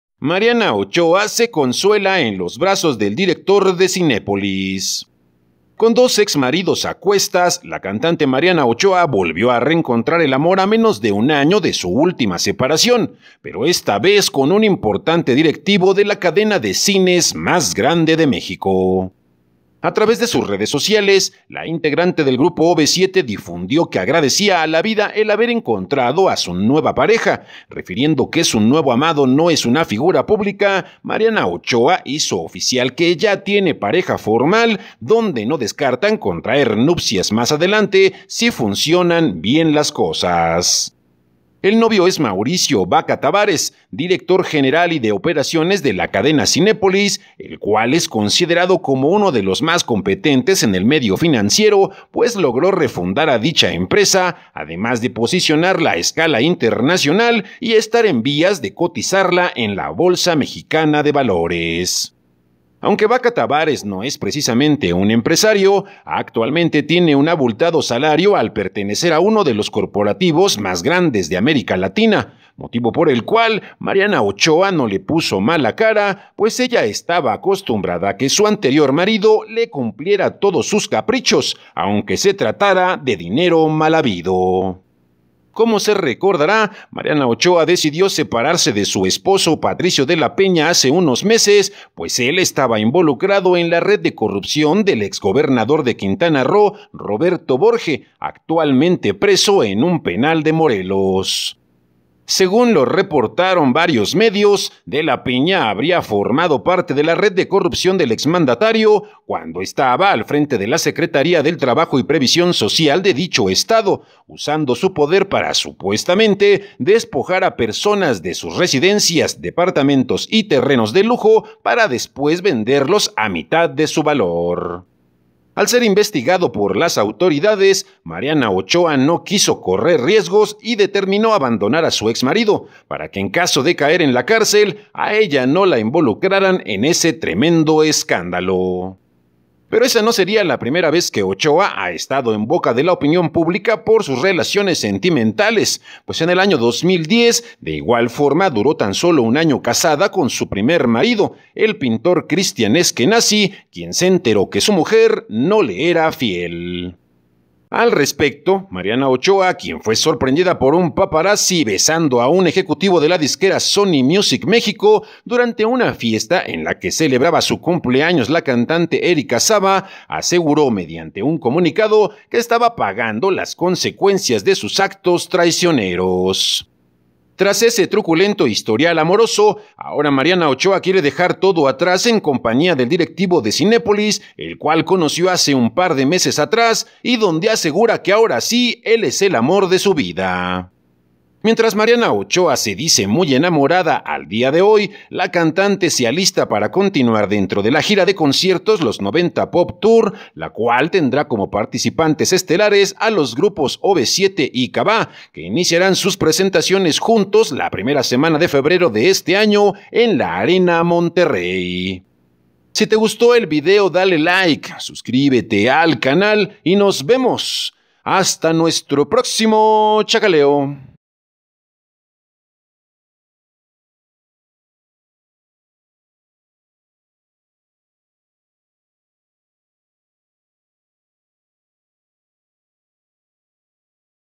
Mariana Ochoa se Consuela en los Brazos del Director de Cinépolis
Con dos exmaridos a cuestas, la cantante Mariana Ochoa volvió a reencontrar el amor a menos de un año de su última separación, pero esta vez con un importante directivo de la cadena de cines más grande de México.